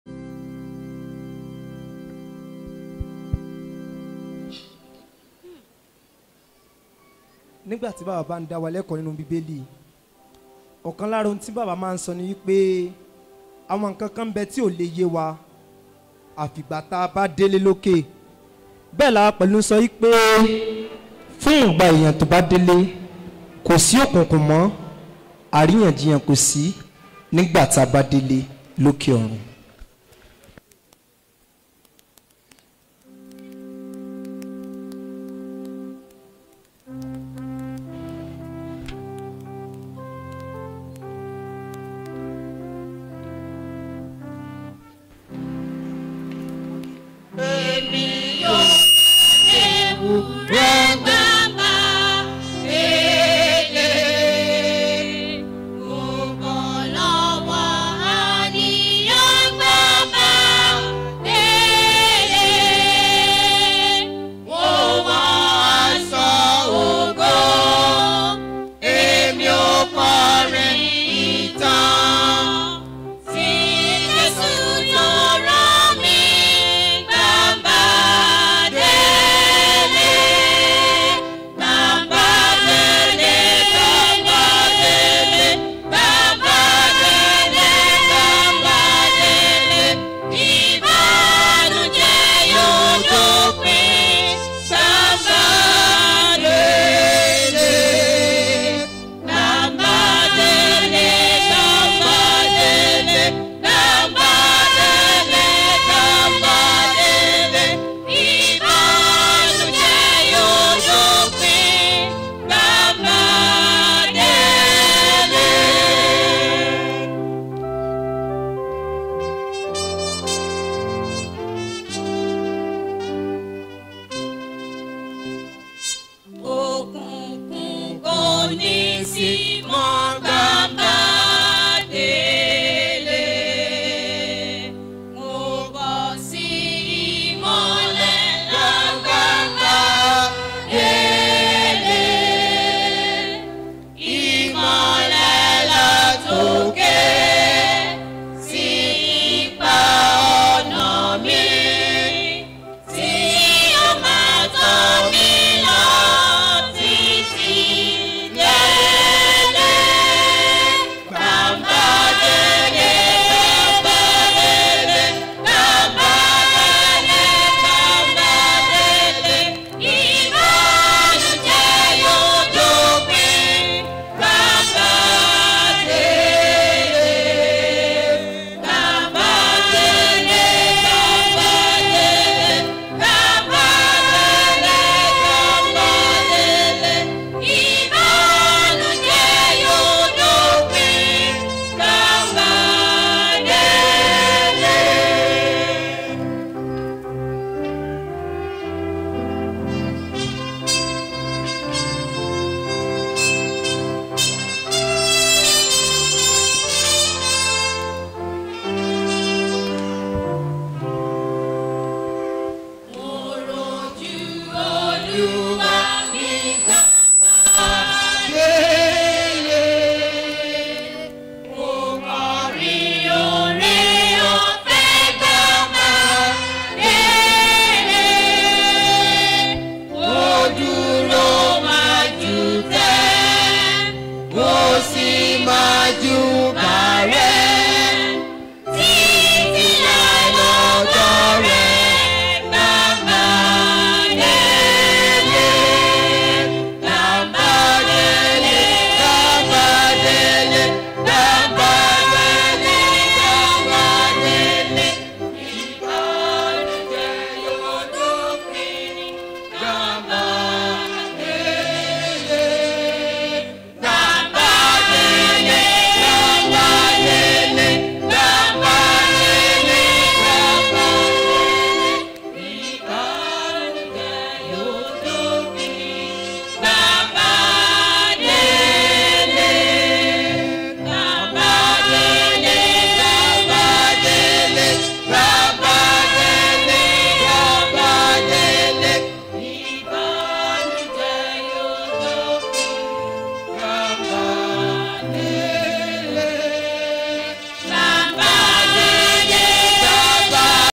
Ayegun Baptist Church Choir, Presentation
Ayegun Baptist Church Choir Presentation at Baba Gomal burial ceremony